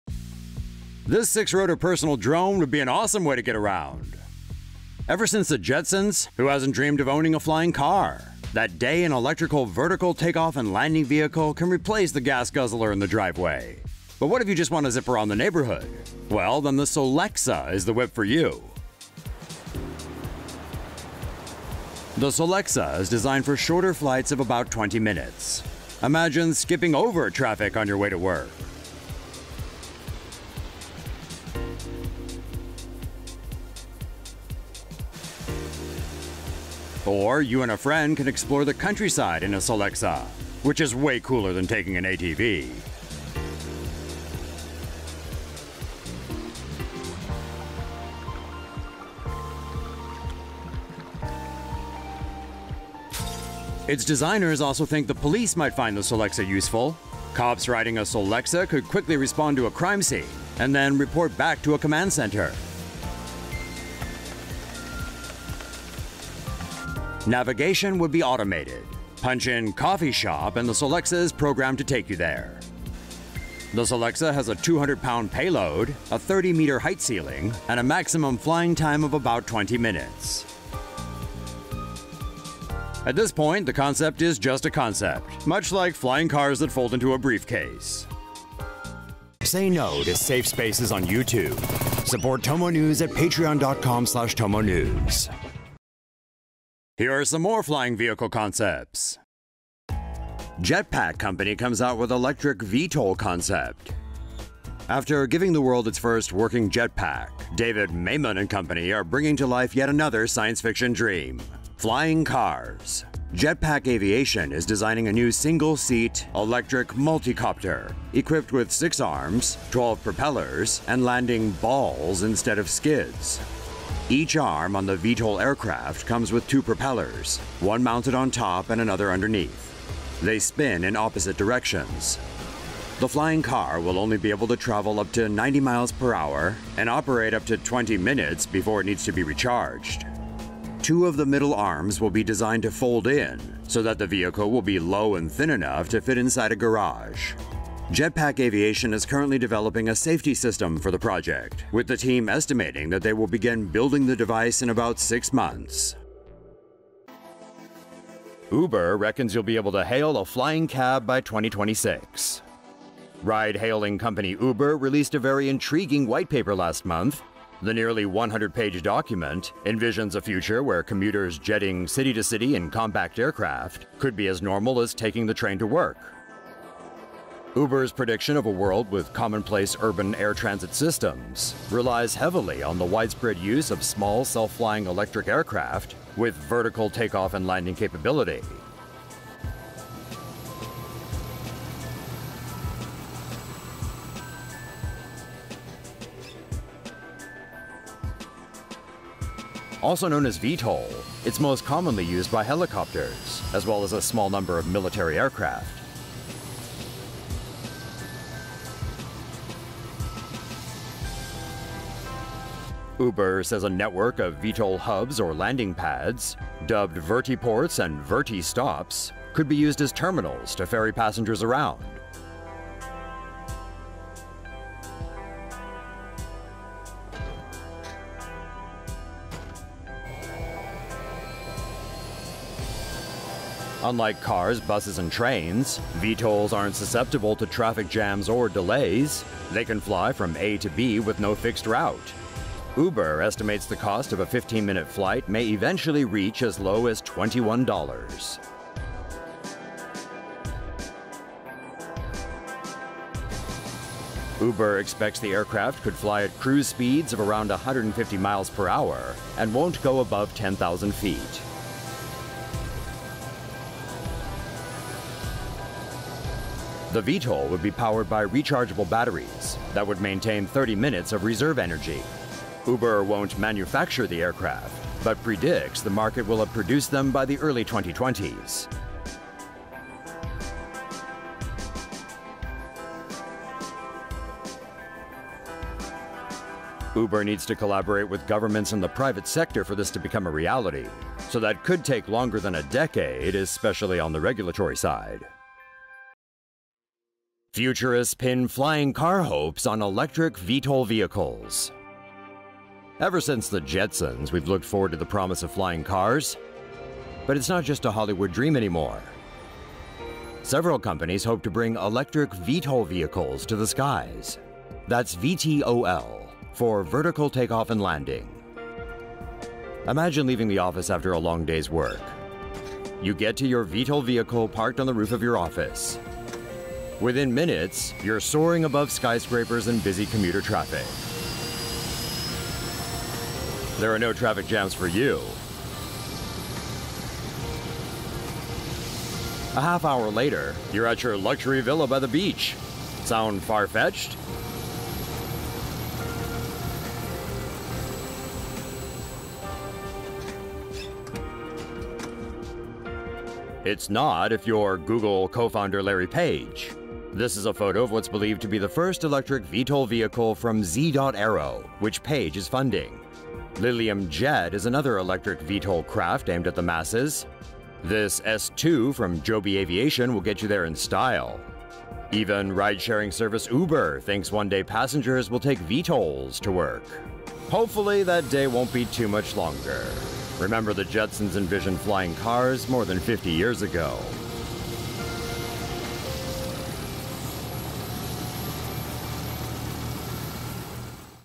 VTOL Aircraft for one: soar over traffic in a Solexa personal flying vehicle - TomoNews
MONTREAL, CANADA — Imagine taking to the skies in your own Jetsons-style personal flying vehicle.

Companies such as Joby Aviation, Zee.Areo and Lilium are developing electric vertical takeoff and landing (VTOL) vehicles, while Uber hopes to build on-demand services around the new form of transportation. The idea is to shorten hours-long highway commutes into a 30-minute door-to-door ride.

Now, technology futurists Charles Bombardier of Imaginative and Pete Bitar of AirBouyant have taken the concept one step further. Their idea: a VTOL designed for quick flights around the neighborhood.

The Solexa looks like a super-sized drone. Yet this one is capable of carrying aloft a 200-pound operator at heights up to 30 meters for as long as 20 minutes. Like other VTOL concepts, navigation could be handled by onboard computers, with operators punching in a destination.

Most VTOL concepts call for ion-lithium batteries. The Solexa would use a compact fuel cell and high octane gasoline to power its six motorized fans.

Bombardier and Bitar think the Solexa would be ideal for short travel across busy city streets or exploring the countryside. Bicycle police might also turn in their rides for a far cooler way to patrol the streets.